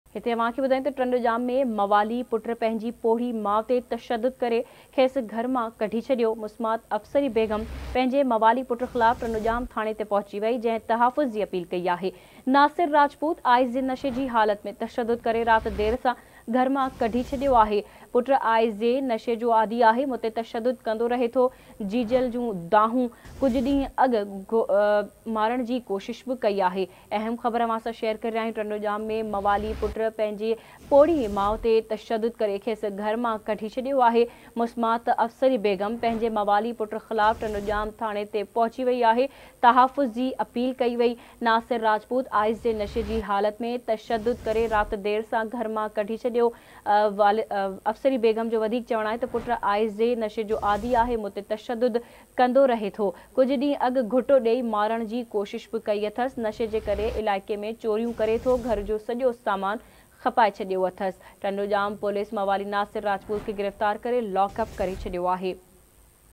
son abused his old mother and threw her out of the house | Breaking | Awaz Tv News
Prime Minister Expressed Regret Shah Noorani accident  | Breaking | Awaz Tv News | Dangerous Accident | Passenger coach overturns  | Breaking | Awaz Tv News